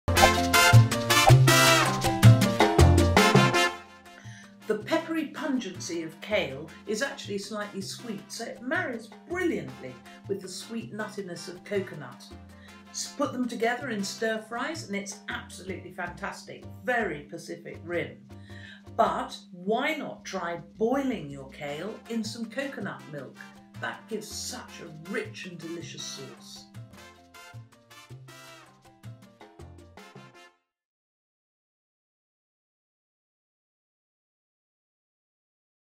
Kale Combinations - Cooking Kale with Coconut
Not sure what flavours work best with kale? We asked the expert and here Rosemary Moon gives her suggestions about the best kale combos around! Here she gives her suggestions about kale and coconut!